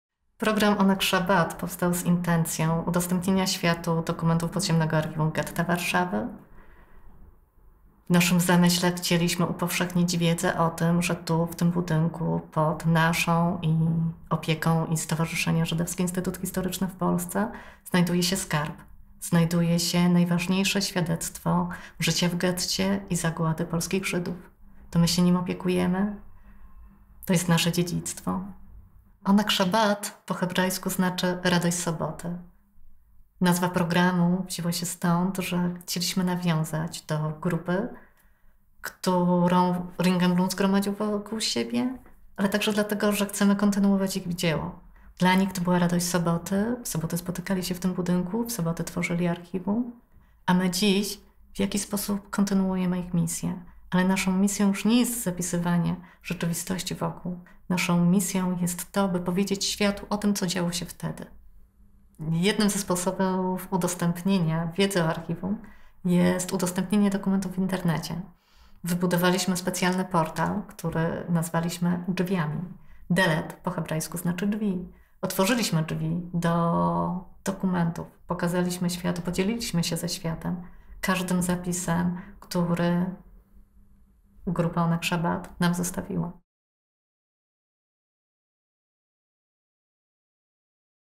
Jolanta Hercog o programie Oneg Szabat oraz portalu Delet
Jolanta Hercog, Zastępczyni Dyrektora ds. Zarządzania Strategicznego w ŻIH opowiada o programie Oneg Szabat oraz portalu Delet.

Delet jest elementem programu Oneg Szabat mającego na celu upowszechnienie spuścizny członków grupy Oneg Szabat: Podziemnego Archiwum Getta Warszawy. Stwarza unikalną okazję nie tylko do wnikliwego zapoznania się z materiałami, ale również do tworzenia na ich podstawie wyjątkowych prezentacji naukowych i edukacyjnych.

Po więcej informacji o programie Oneg Szabat zapraszamy na stronę Zapraszamy również na stronę portalu Delet

Filmy oraz tłumaczenia materiałów na wystawę powstały przy wsparciu finansowym Jewish Federations of Canada - UIA w ramach programu Marsz Żywych.